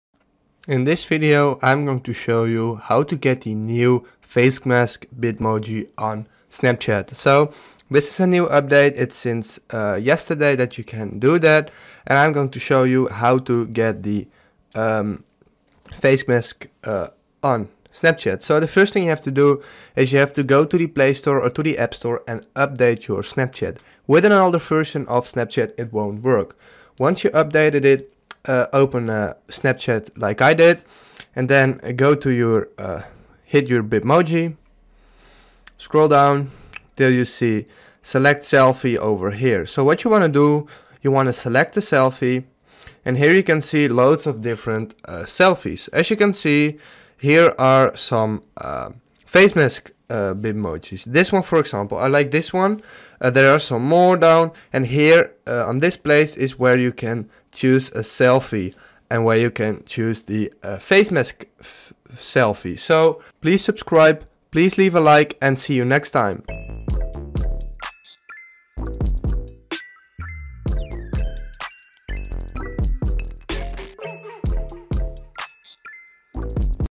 How to Get The Face Mask Bitmoji on Snapchat!
In this video I'll show you how to get the face mask bitmoji 2020. The method is very simple and clearly described in the video. Follow all of the steps in the video, and get the face mask bitmoji.

1. go the play / appstore
2. update snapchat
3. go to snapchat
4. select the mask

That's how you can get the face mask on snapchat 2020!